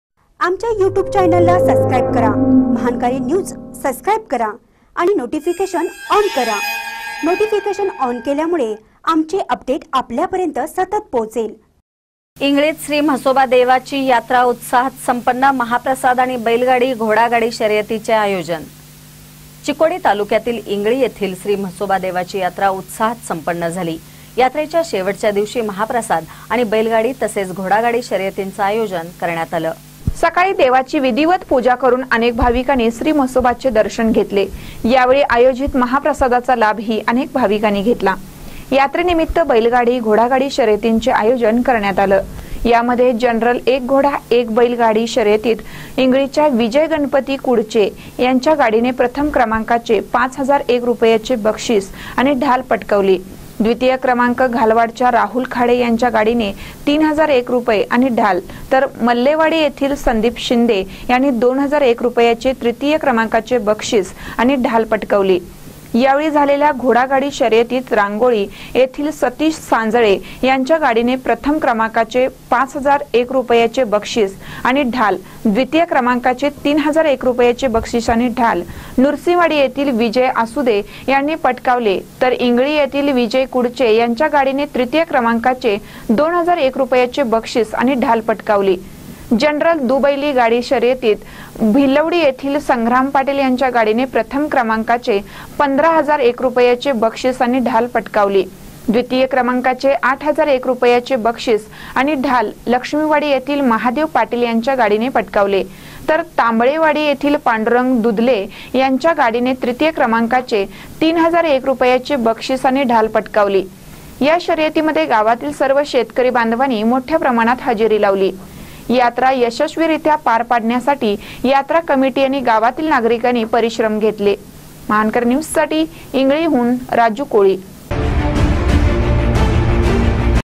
इंगळीत श्री म्हसोबा देवाची यात्रा उत्साहात संपन्न
इंगळीत श्री म्हसोबा देवाची यात्रा उत्साहात संपन्न : महाप्रसाद आणि बैलगाडी, घोडागाडी शर्यतीचे आयोजन